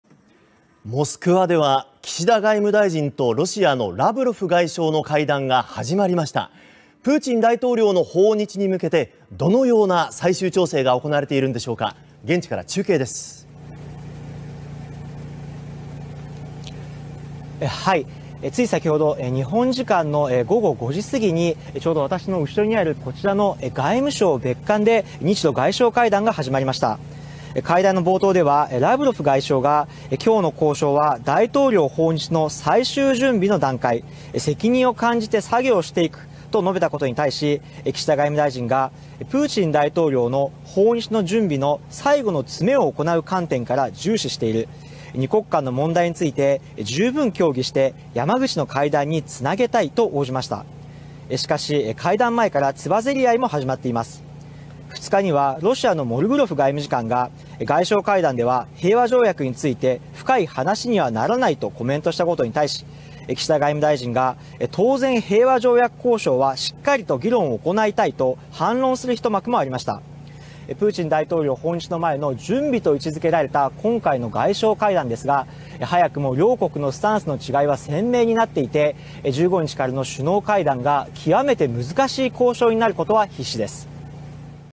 「大統領訪日の最終準備の段階」日ロ外相会談始まる(16/12/03)
モスクワでは、岸田外務大臣とロシアのラブロフ外相の会談が始まりました。プーチン大統領の訪日に向けて、どのような最終調整が行われているのでしょうか。　（長谷川由宇記者報告）　日本時間の3日午後5時すぎに、外務省別館で日ロ外相会談が始まりました。